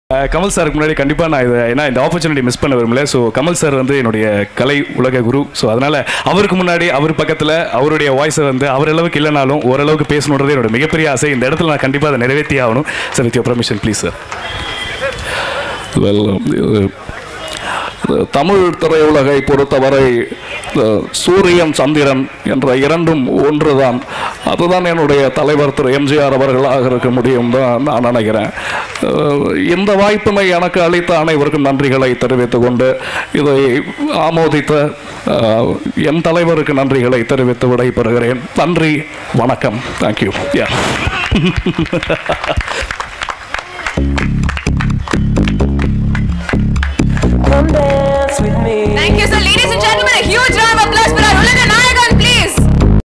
கமல்ஹாசனின் முன்பு பேசி அசத்திய Mimicry Naveen
Naveen Rasigar Mandram Pondicherry